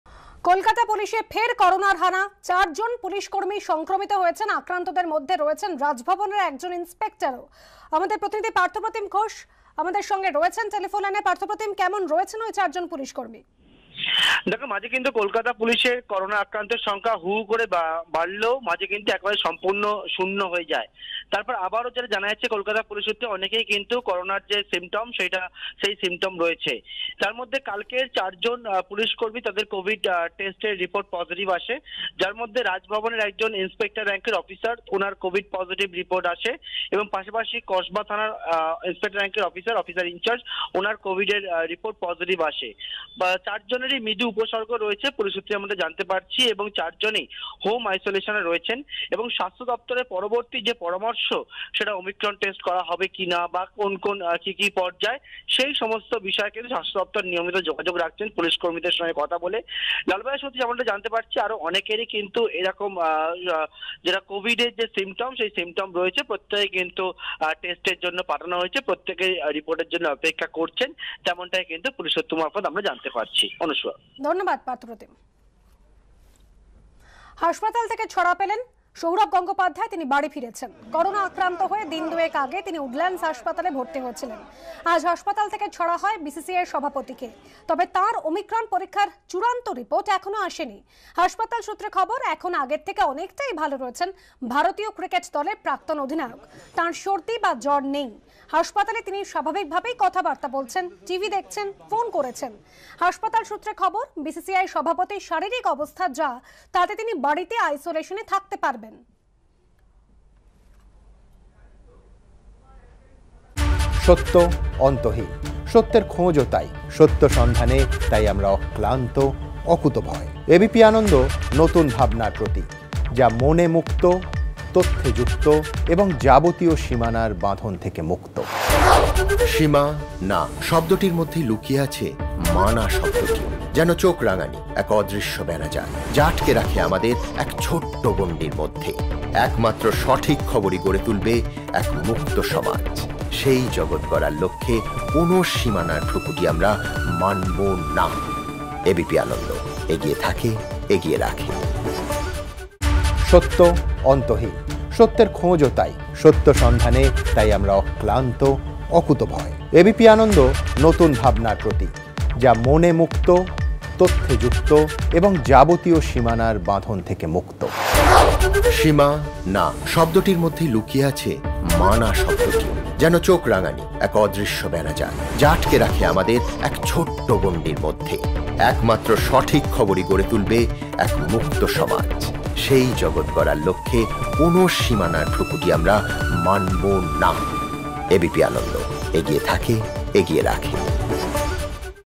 Covid Update: কোভিড আক্রান্ত কলকাতা পুলিশের ৪ কর্মী, রয়েছেন হোম আইসোলেশনে| Bangla News
কলকাতা পুলিশে ফের করোনার হানা। ৪ পুলিশকর্মী সংক্রমিত।আক্রান্তদের মধ্যে আছেন রাজভবনের এক ইন্সপেক্টর।আক্রান্তদের মধ্যে আছেন কসবা থানার ওসি-ও। সব আক্রান্তদের আছে মৃদু উপসর্গ।প্রত্যেকেই রয়েছেন হোম আইসোলেশনে।